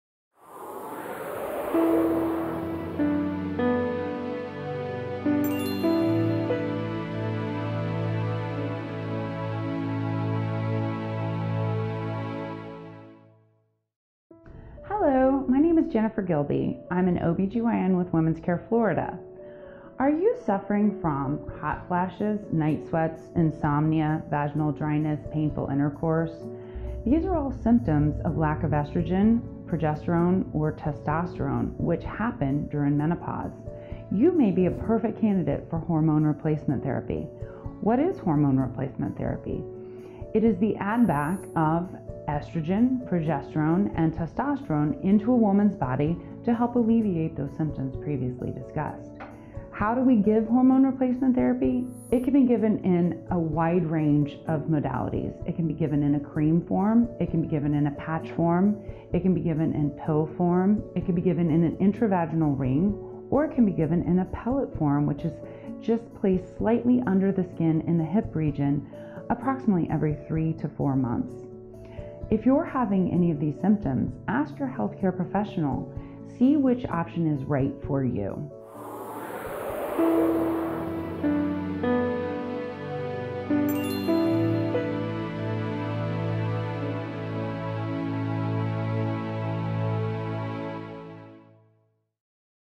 Hormone Replacement Therapy
Dr. Gilby explains some of the symptoms resulting the lack of certain hormones related to menopause, as well as the treatment options often used to remedy these symptoms.